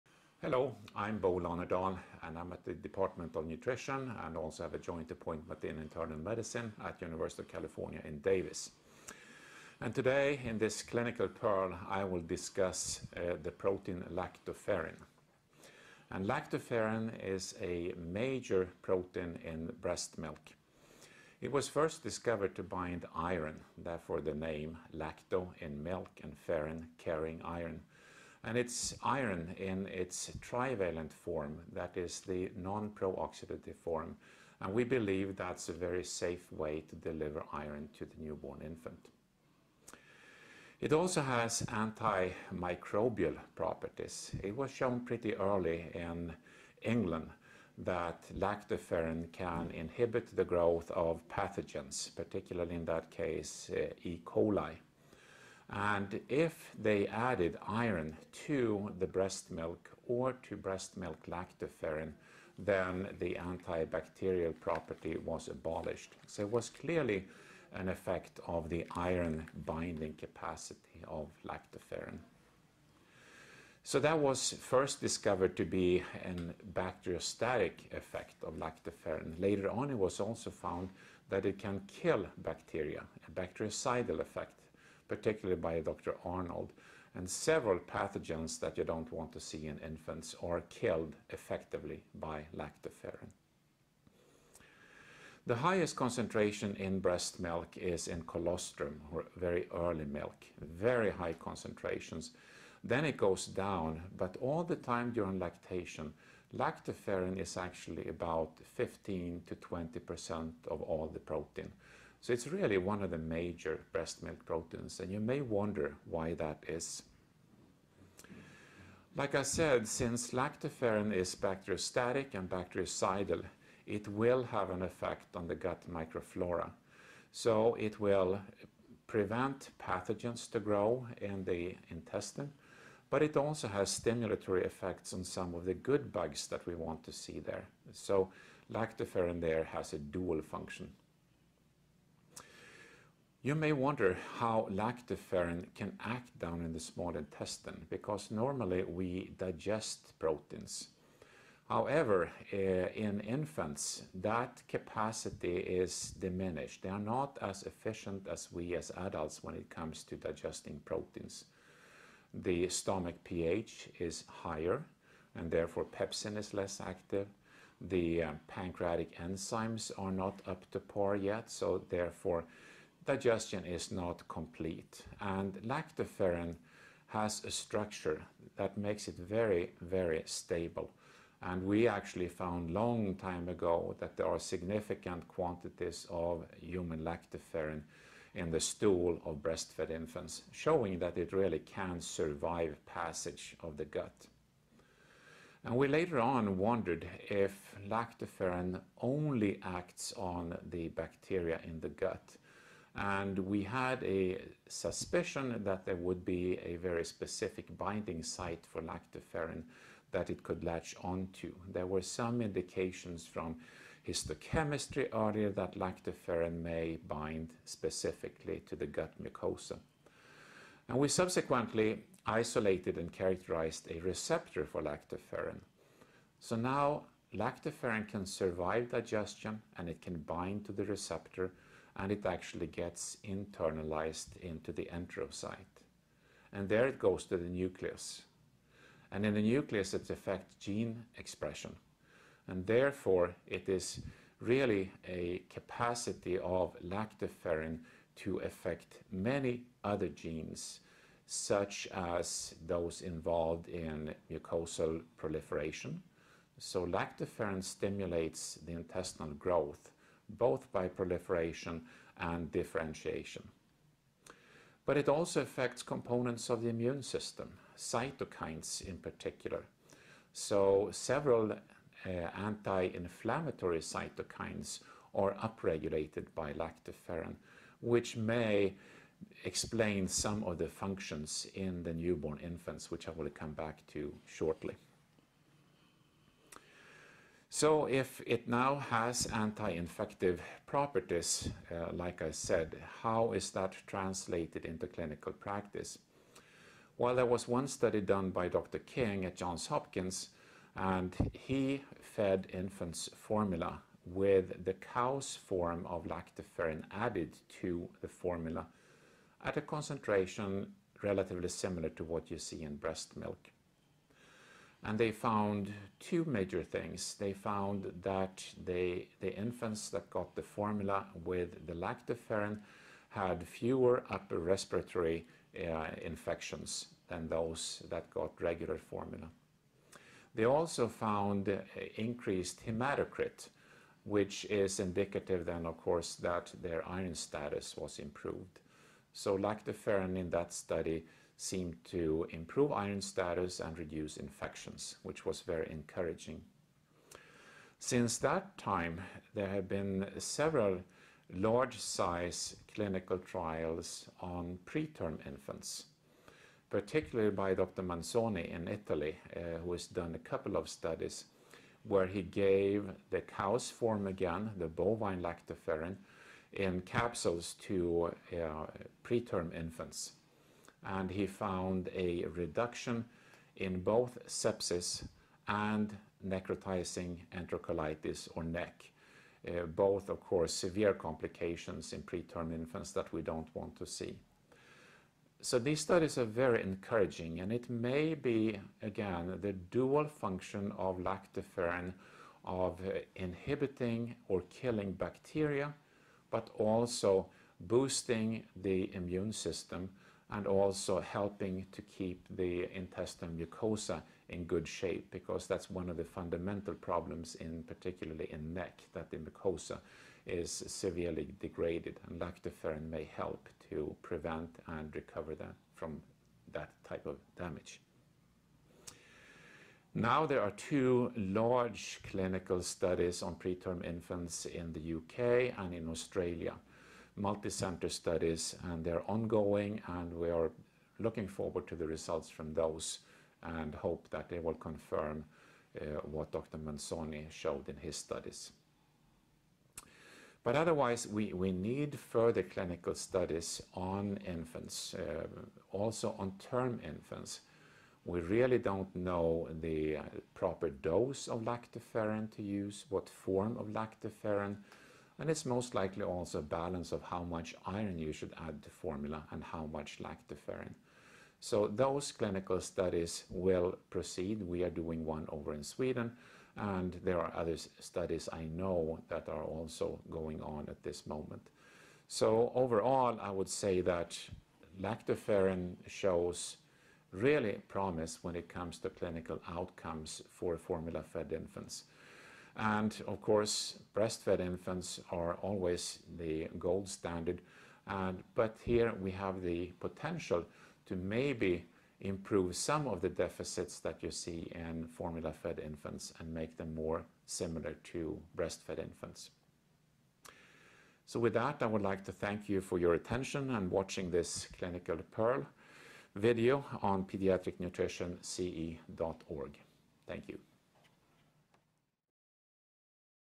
Lactoferrin Supplementation and its Gut Health Benefits
Bo Lönnerdal, PhD, Distinguished Professor Emeritus in the Department of Nutrition and the Department of Internal Medicine at the University of California, Davis, discusses the current state of the science of lactoferrin as a nutritional supplement. Dr. Lönnerdal describes the antimicrobial properties of lactoferrin and how this is essential for shaping the composition of the microbiota, and he suggests how lactoferrin supplementation can improve outcomes for preterm infants.